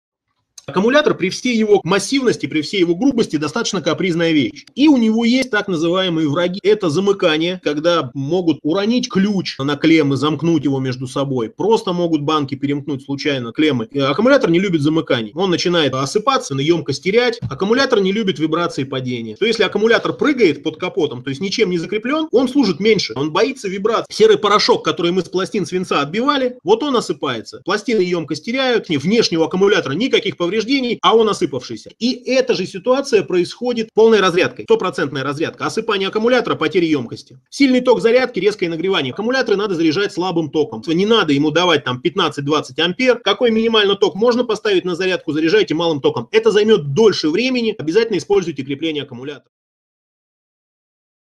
Ошибки, которые убивают ваш аккумулятор Daewoo Matiz 0.8L-1.0L МКПП и АКПП
на вопрос отвечает Владимир Труфанов
Вебинар Мир Матизов “Зимняя эксплуатация Matiz"